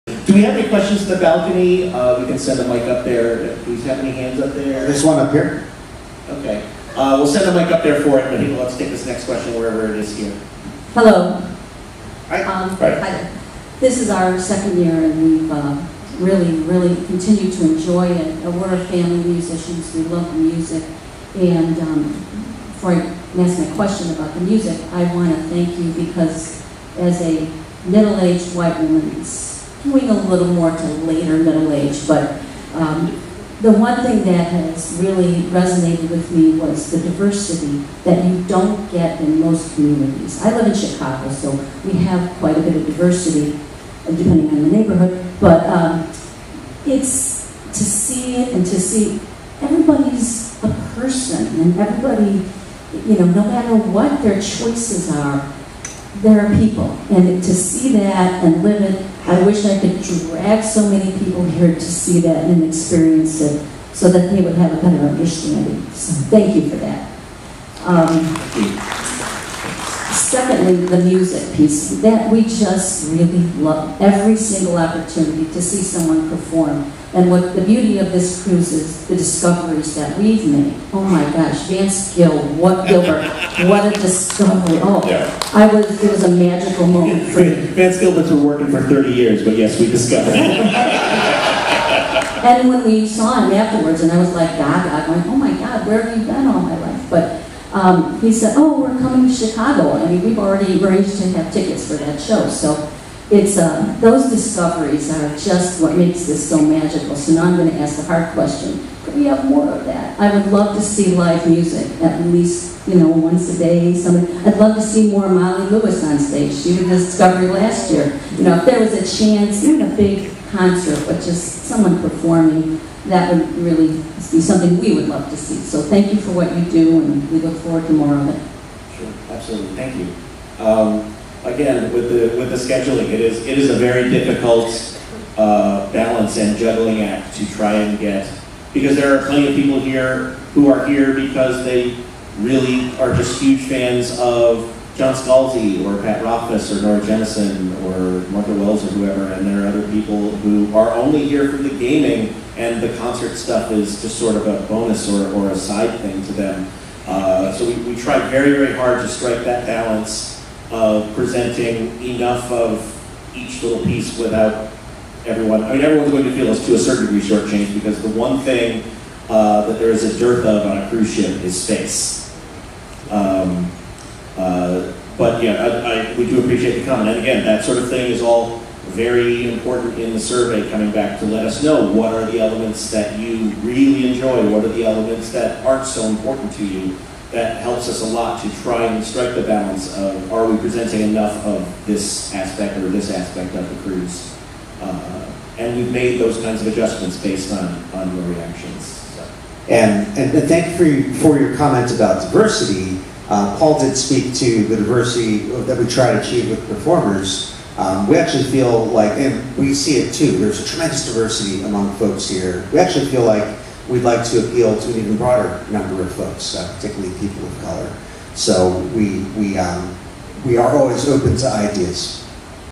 Yay for diversity, and can we have more music? — JoCo Cruise 2020 Management Q&A
Someone thanks the Management for the diversity in the guests and performers here, and asks if we can have more music.